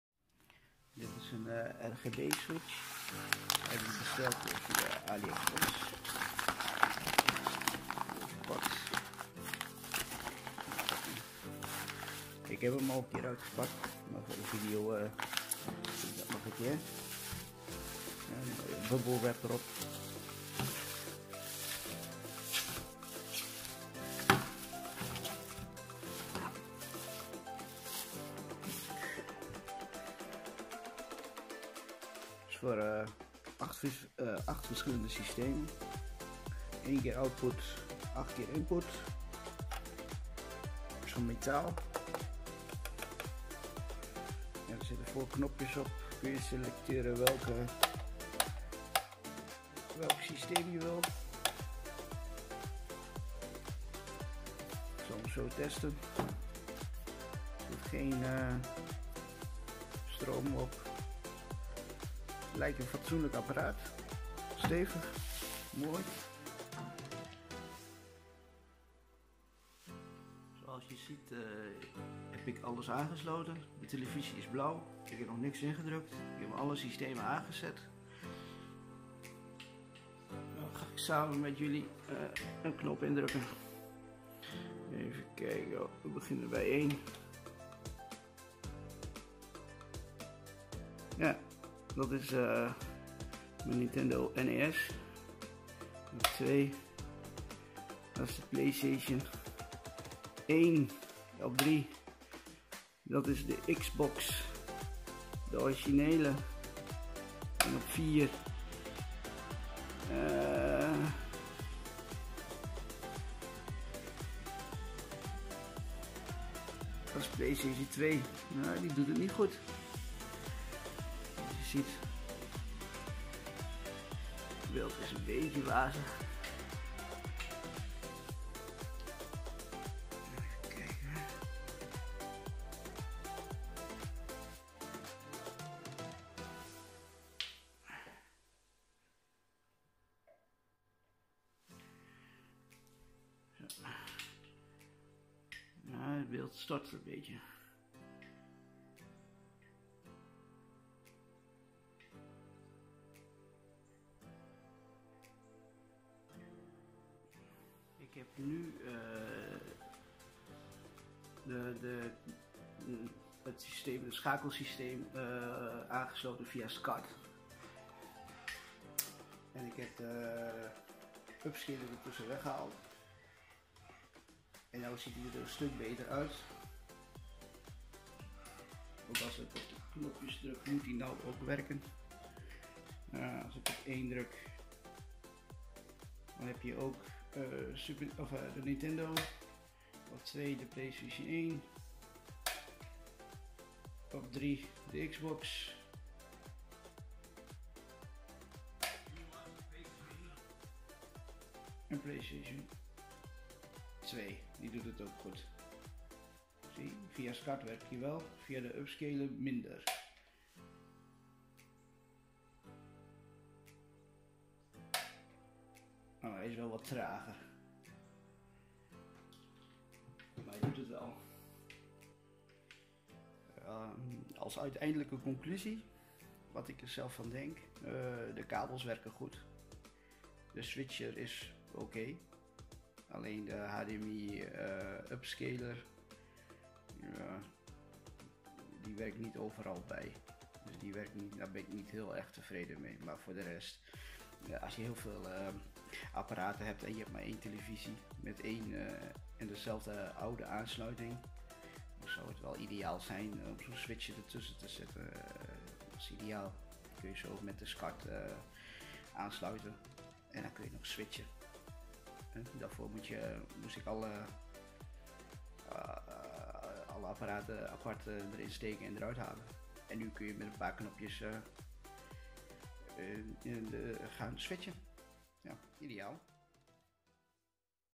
8 channels av switch for retro consoles(NL)
Uitpakken en testen van de switcher box van aliexpress

SmileMango 8 Ports Composite 3 RCA Video Audio AV Switch Switcher Box Selector 8 In 1 Out 8x1 for HDTV LCD Projector DVD